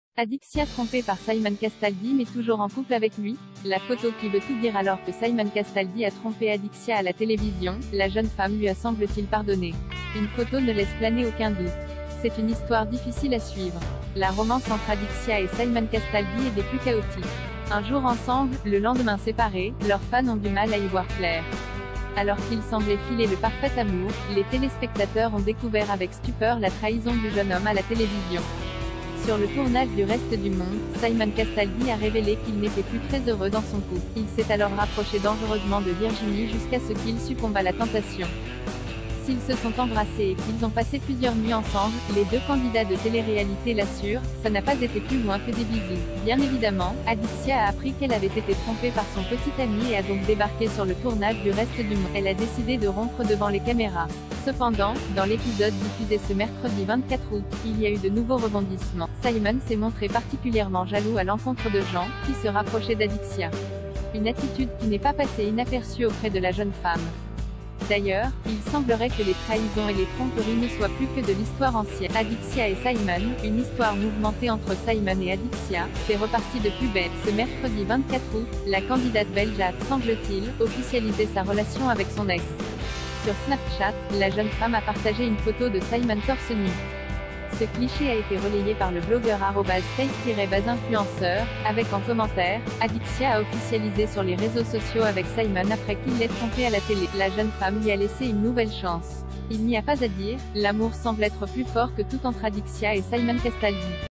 Adixia trompée par Simon Castaldi mais toujours en couple
Adixia trompée par Simon Castaldi mais toujours en couple avec lui ? La photo qui veut tout dire  Alors que Simon Castaldi a trompé Adixia à la télévision, la jeune femme lui a semble-t-il pardonné. Une photo ne laisse planer aucun doute.    C’est une histoire difficile à suivre. La romance entre Adixia et Simon Castaldi est des plus chaotiques. Un jour ensemble, le lendemain séparés, leurs fans ont du mal à y voir clair. Alors qu’ils semblaient filer le parfait amour, les téléspectateurs ont découvert avec stupeur la trahison du jeune homme à la télévision. Sur le tournage du Reste du Monde, Simon Castaldi a révélé qu’il n’était plus très heureux dans son couple. Il s’est alors rapproché dangereusement de Virginie jusqu’à ce qu’il succombe à la tentation. S’ils se sont embrassés et qu’ils ont passé plusieurs nuits ensemble, les deux candidats de téléréalité l’assurent, ça n’a pas été plus loin que des bisous.     Bien évidemment, Adixia a appris qu’elle avait été trompée par son petit ami et a donc débarqué sur le tournage du Reste du Monde. Elle a décidé de rompre devant les caméras. Cependant, dans l’épisode diffusé ce mercredi 24 août, il y a eu de nouveaux rebondissements. Simon s’est montré particulièrement jaloux à l’encontre de Jean, qui se rapprochait d’Adixia. Une attitude qui n’est pas passée inaperçue auprès de la jeune femme. D’ailleurs, il semblerait que les trahisons et les tromperies ne soient plus que de l’histoire ancienne. Adixia et Simon, une histoire mouvementée  Entre Simon et Adixia, c’est reparti de plus belle. Ce mercredi 24 août, la candidate belge a, semble-t-il, officialisé sa relation avec son ex. Sur Snapchat, la jeune femme a partagé une photo de Simon torse-nu. » La jeune femme lui a laissé une nouvelle chance. Il n’y a pas à dire, l’amour semble être plus fort que tout entre Adixia et Simon Castaldi.